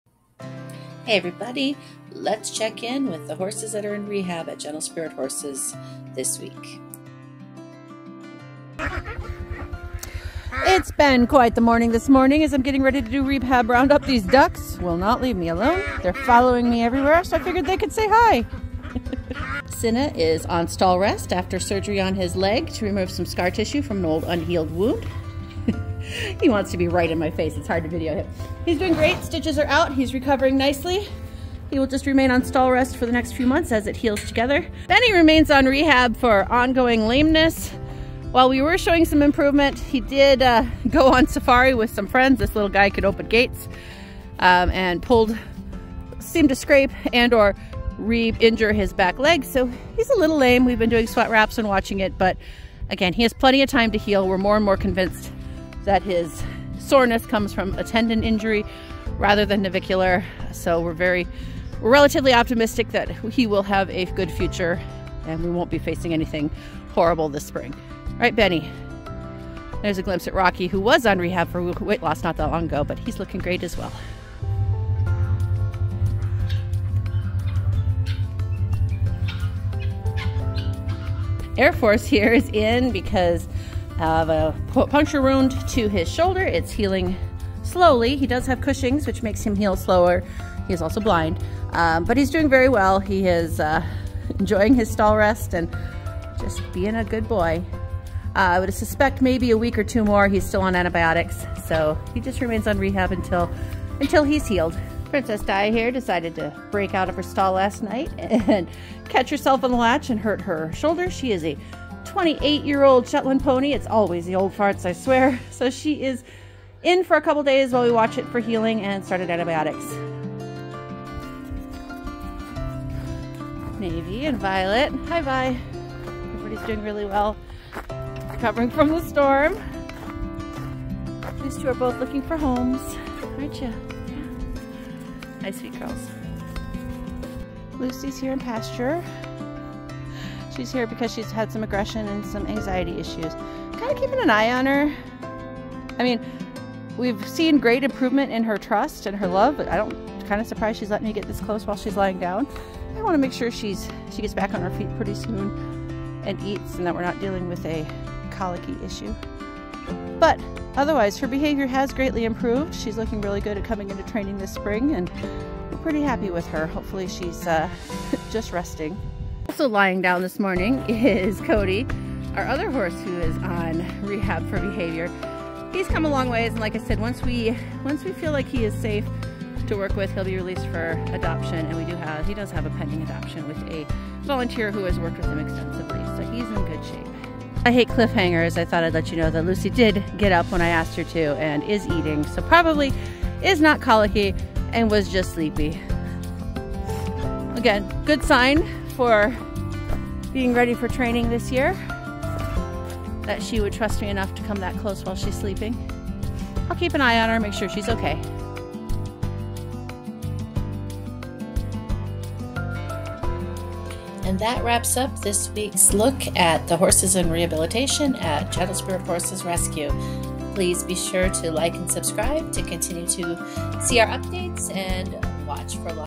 Rehab Roundup Jan 24, 2024 - let's check in on the horses in rehab at Gentle Spirit Horses Rescue
RehabRoundup January 24, 2024 - let's check in on the horses currently in rehab at Gentle Spirit Horses Rescue near Scotland, South Dakota

Medical

Benny - 16-yr-old TB gelding, navicular/lameness. Has been showing improvement, however one of his friends opened a gate and they went on safari through the snow drifts and he seems to have reinjured his leg. We've been wrapping and will continue to give him time.

Cinna - 11/11 auction save, 5-yr-old QH gelding, swollen/injured leg. X-rays showed no bone or joint injury, just a large untreated wound. Had surgery on 1/3 to open the wound and remove scar tissue to encourage healing and decrease the size of the leg.

Airforce - 18-yr-old blind gelding. In rehab for a puncture wound on shoulder he sustained during the blizzard, likely when we had to combine two paddocks due to water freezing issues. On antibiotics for 10 days and stall rest until healed.

Princess Di - 29-yr-old Shetland Pony mare (I said 28 in the video because we are still getting used too switching their ages as of Jan 1, but she is 29). Spooked and broke out of her stall and cut her side. Antibiotics and stall rest until healed.

 Behavior

Lucy - in pasture, calm and friendly. Much calmer when bonded friends removed from pasture and accepting attention even in a group of friends without aggression. Has been accepting the halter over her nose and not showing any aggression when her friends are removed and actively seeking attention from people. Goal: Dakota Showdown 2024 prospect.

Kody - in pasture, calming, and curious. Physical condition fully rehabbed, working on friendliness. Is being caught and brought in, and had his first (lightly sedated) meeting with the farrier.

Support the Rehab Horses

Help support our rehab horses by sending them something from our Amazon Wishlist!